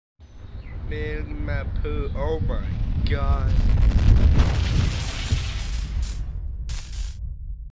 Satellite crash into backyard
Random satellite crashed into our backyard and ruined our inflatable pool :(
Wtf.